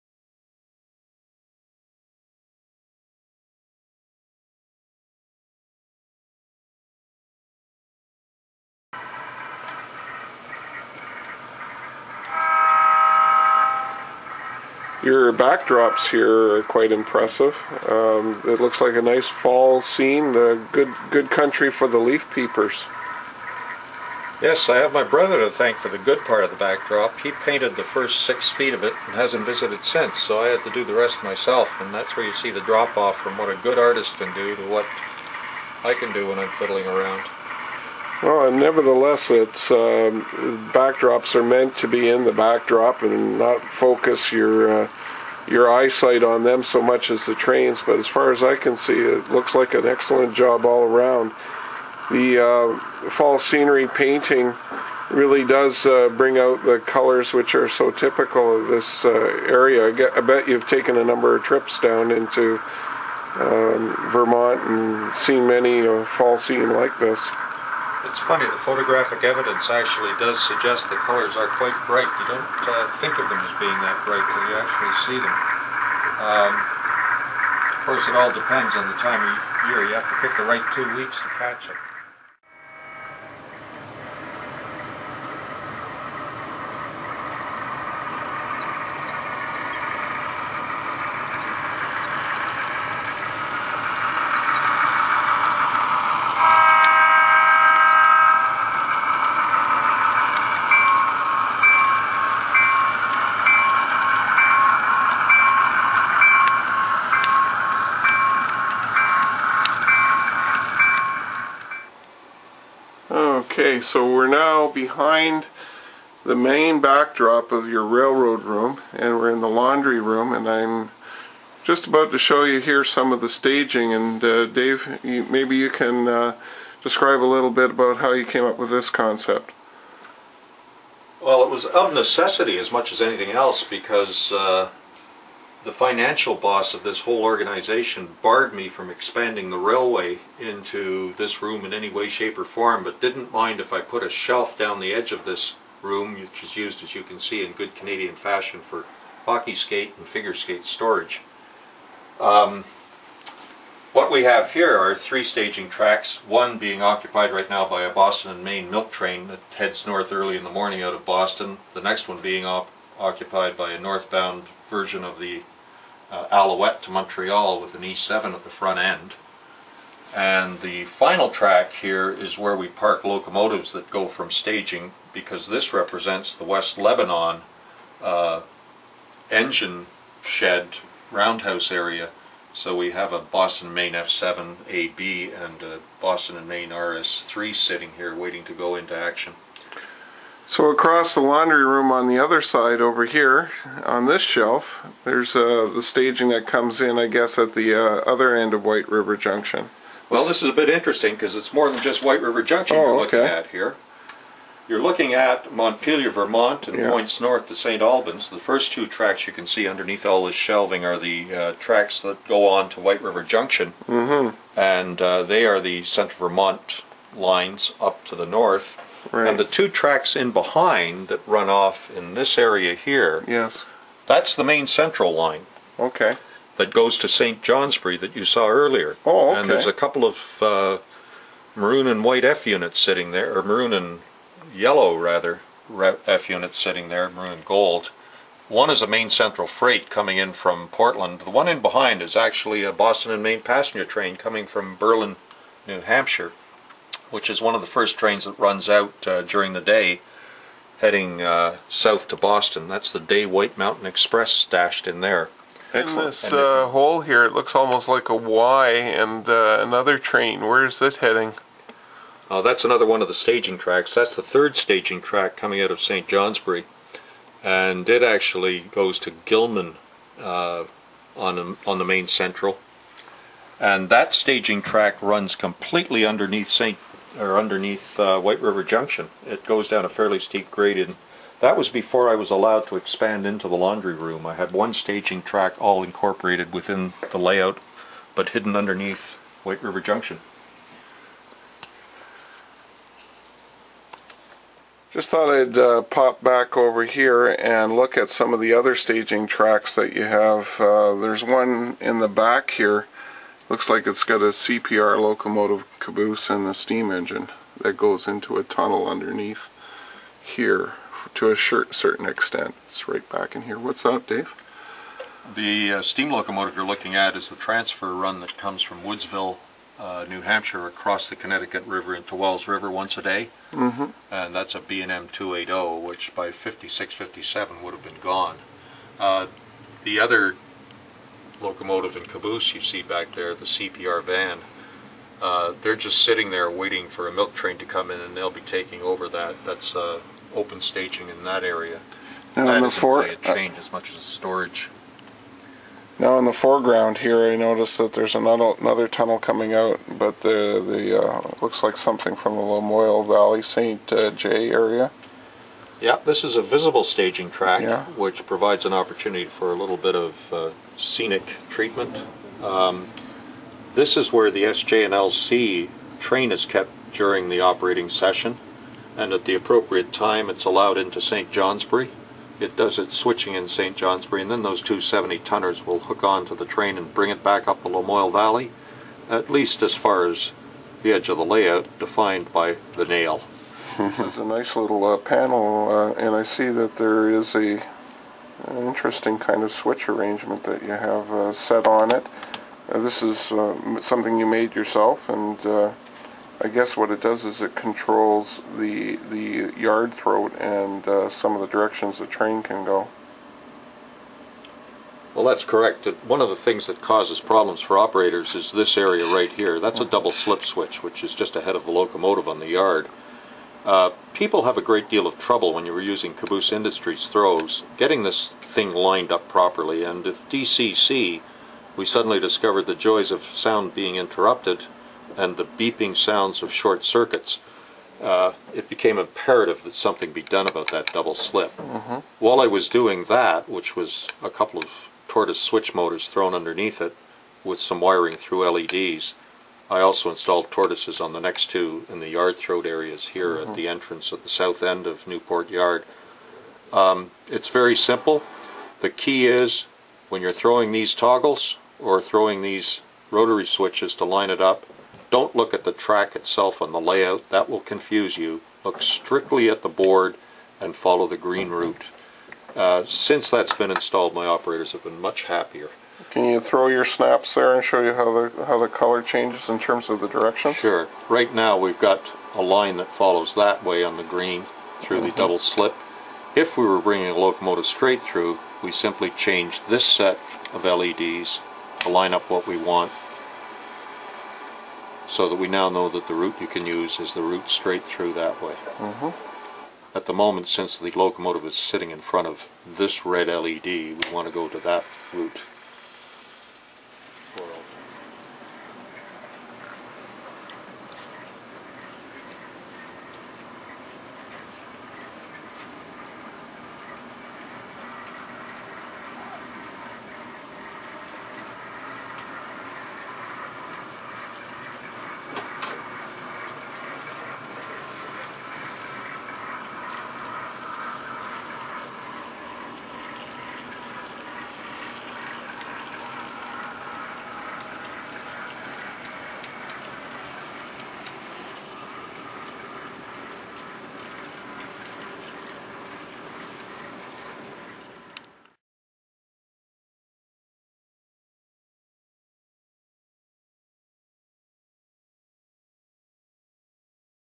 Dave Primeau: Part 5 of 6: Scenery, Backdrops, Staging and Double Slip.
In Part 5, Dave discusses the scenic inspiration for his fall colours and takes us on a tour which includes some inventive staging techniques and improved operation of a double slip switch which use to confuses operators on his model railroad.